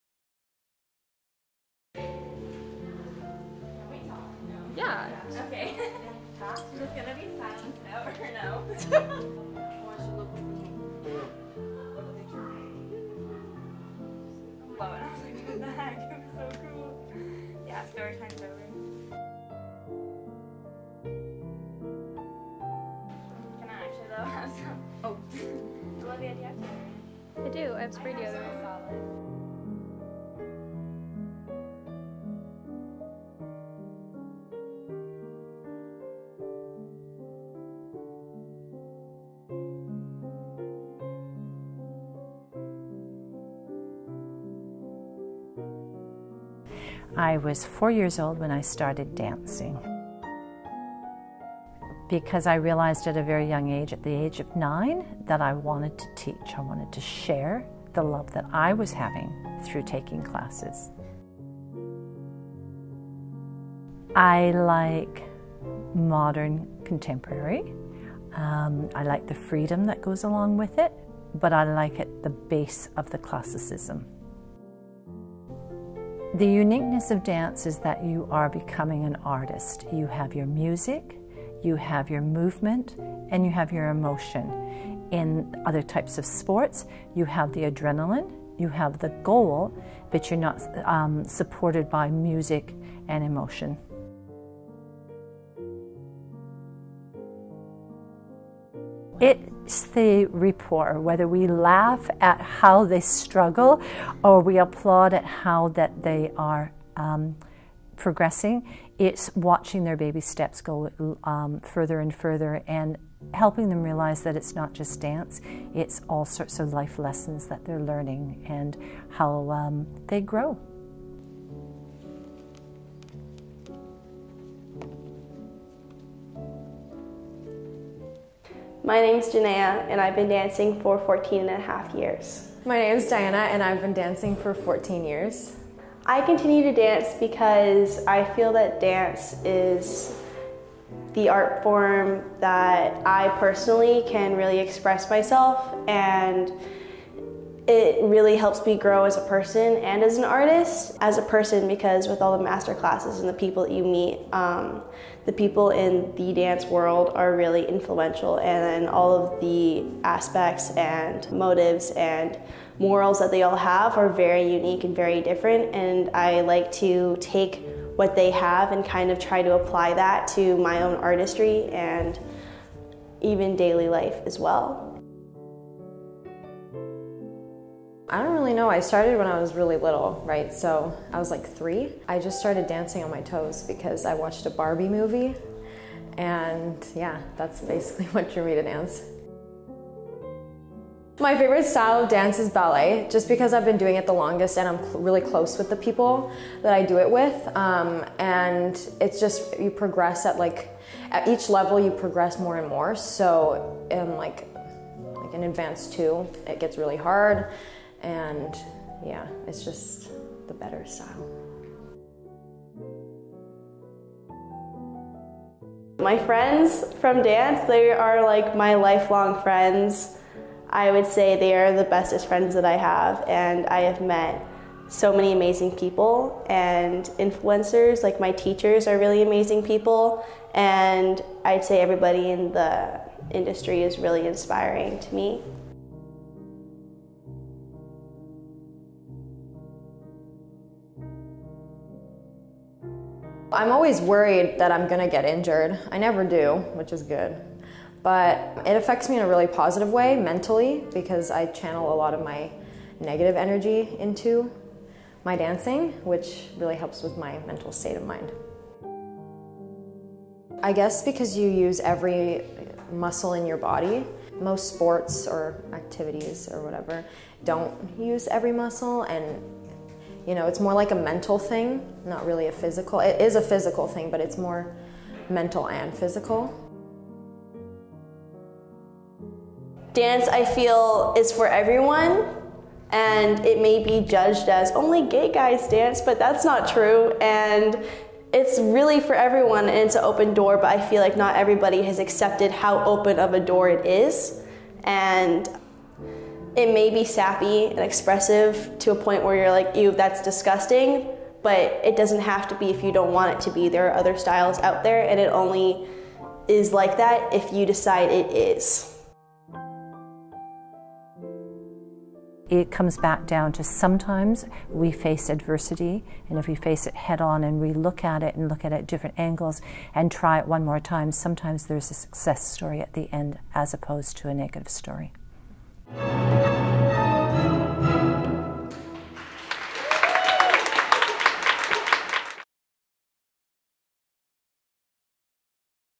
What's The Pointe Of It Documentary - Olivia Scotcher
this is a project that I made for school in grade 11 and is based on how dance impacts the lives of those who participate in it.

filmed at Cameron Dance Academy